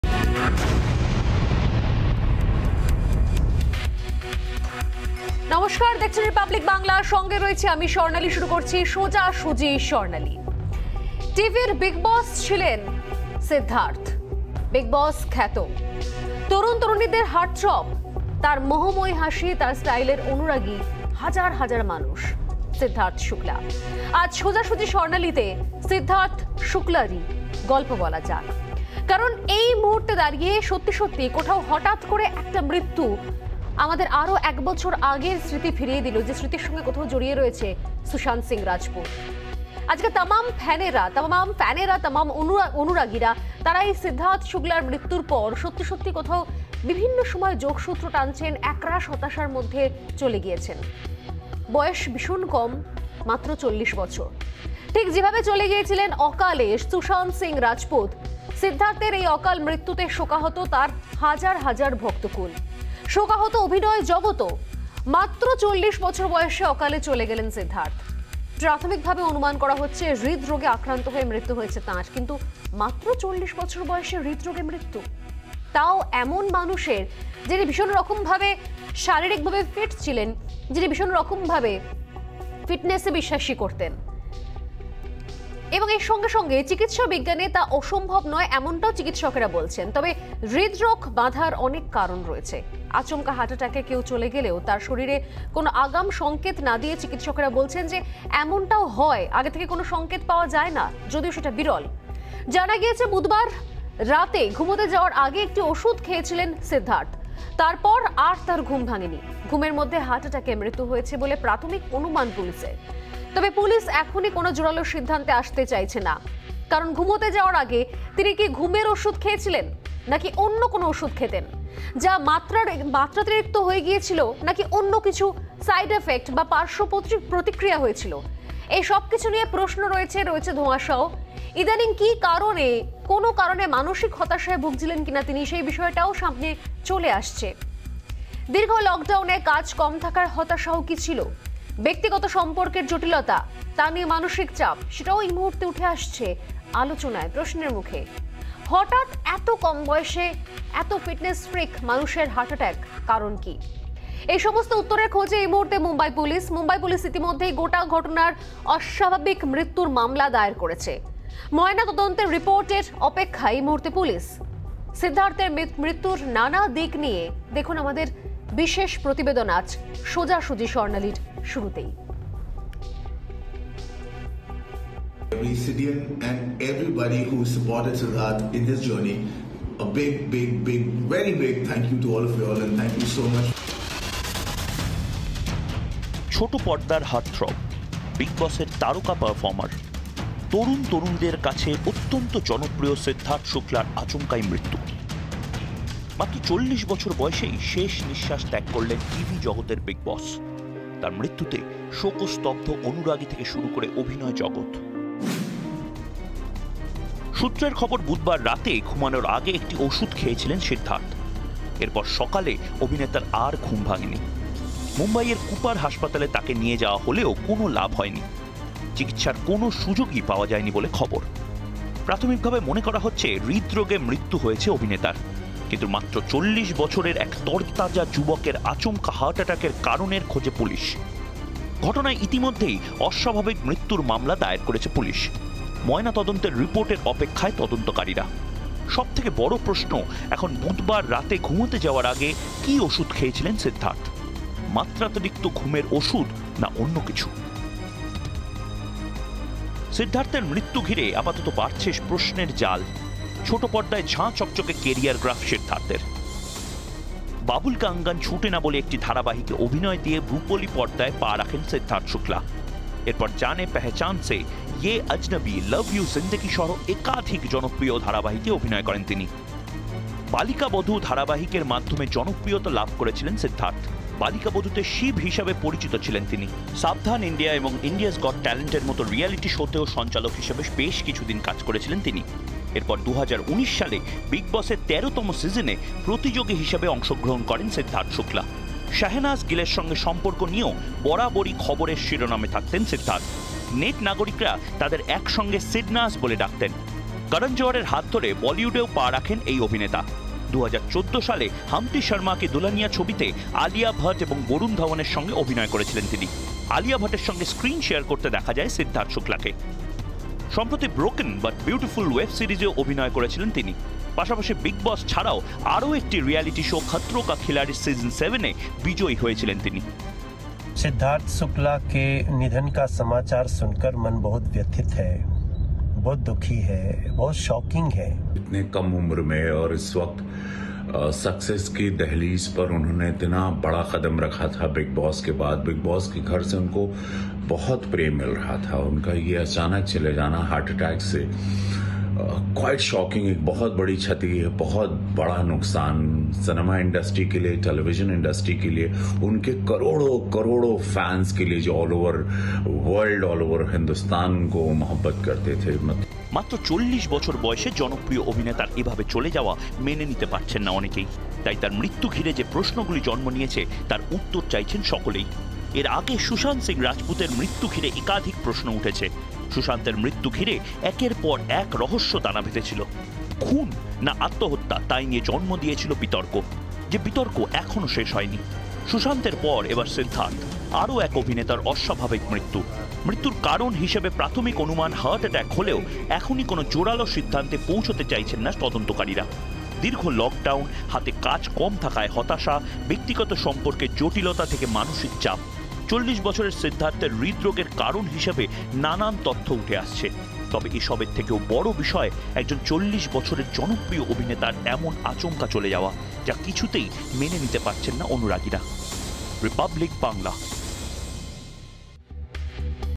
প্রয়াত সিদ্ধার্থ শুক্লা। মাত্রাতিরিক্ত ঘুমের ওষুধে মৃত্যু? নাকি চাপ, হতাশা থেকে হৃদরোগ? উঠছে প্রশ্ন
রিপাবলিক স্বাধীন।
রিপাবলিক নির্ভীক।
আপনিই রিপাবলিক।